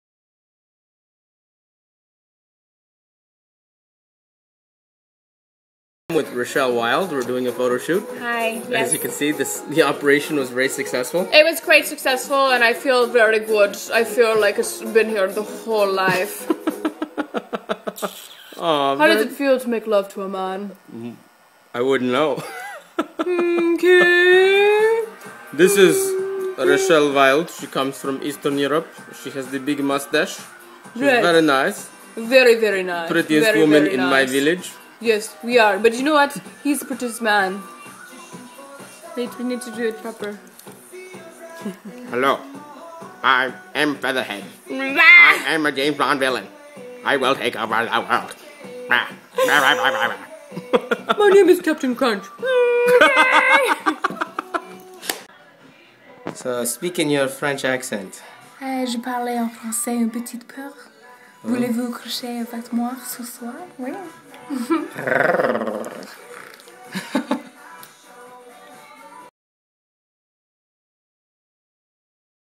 Rachelle Wilde Fun
A few moths ago I did a shoot with Playboy cover model Rachelle Wilde. WE made a bunch of goofy videos these two were the funniest. So I put them together with Splice on my iPhone. Rachelle is so much fun to work with.